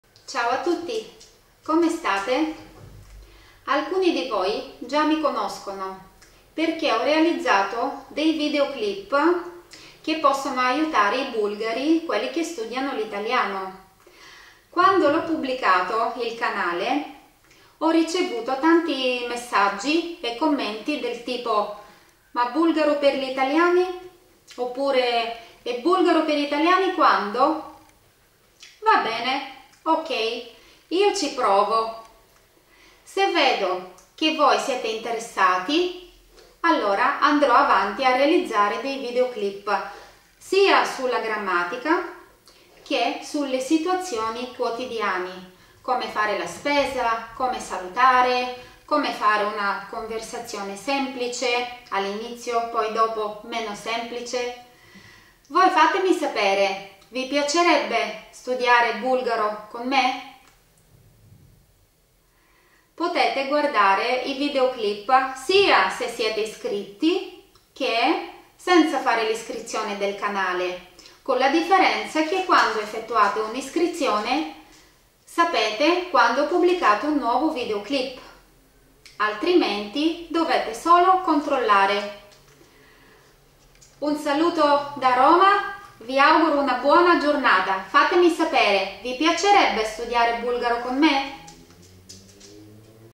Vi piacerebbe studiare il bulgaro con me?
Ho fatto il video su richiesta, fatemi sapere e vi aiuto io :-)
I video clip sono realizzati come un qualcosa in più per aiutarvi a seguire il vostro programma nell'apprendimento della lingua bulgara.